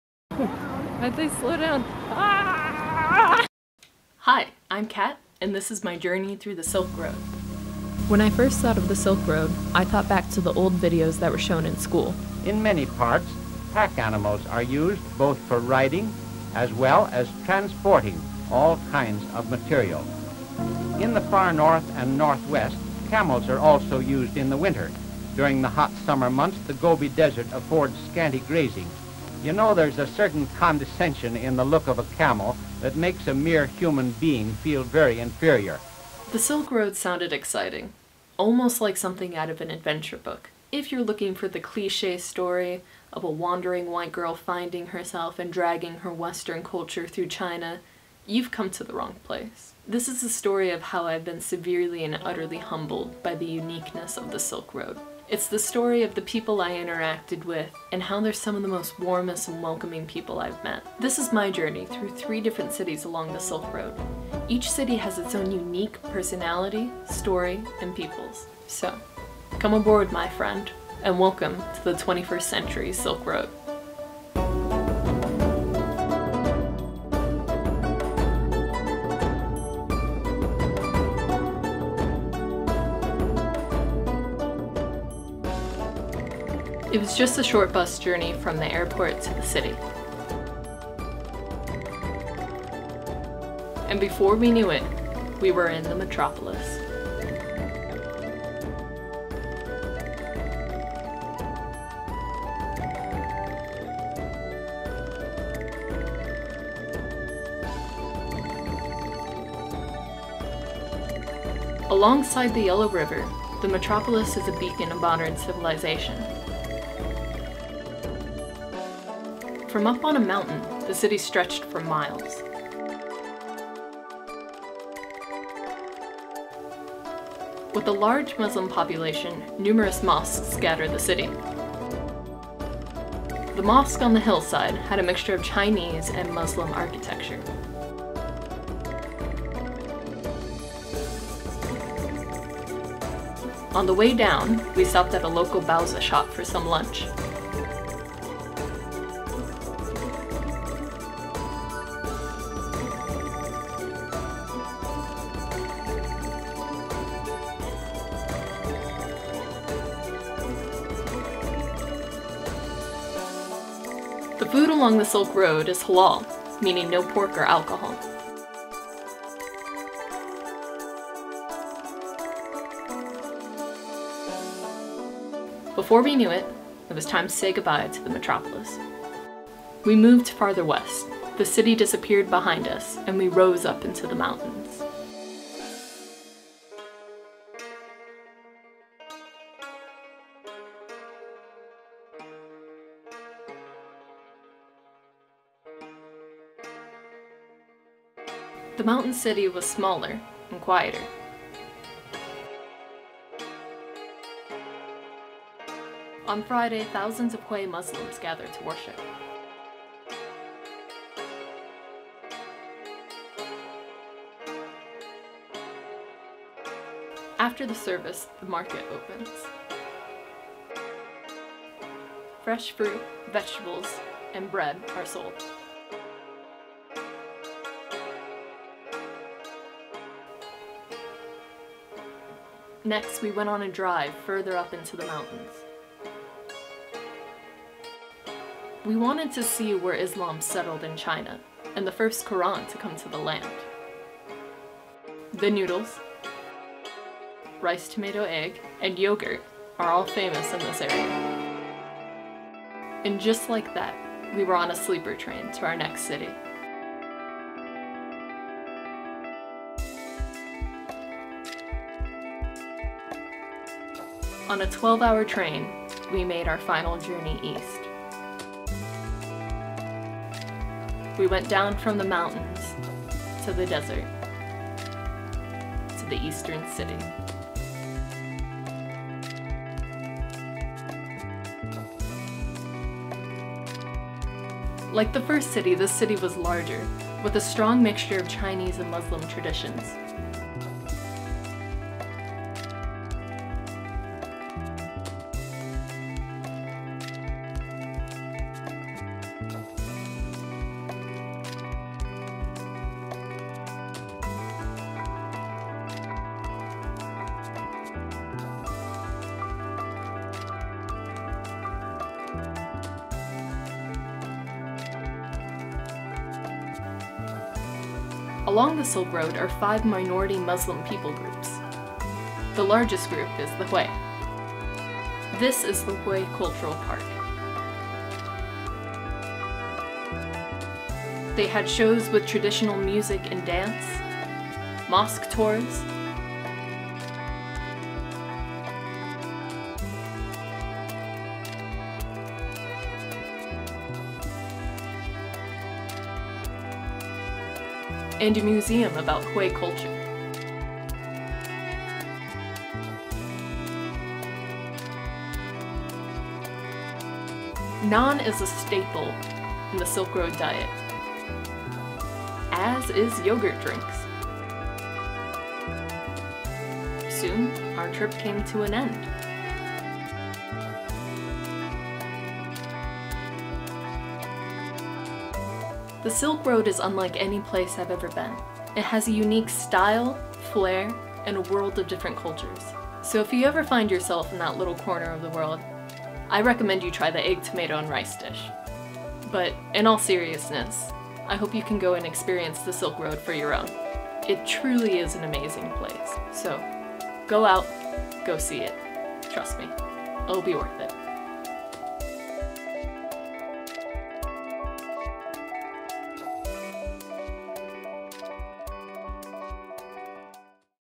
A Journey Through the Silk Road
Join Cat on her journey along China's Silk Road exploring three very different cities and learning more about the people who call this region home.

Want to explore the area yourself? Take a look at our Silk Road prayer journeys to find out more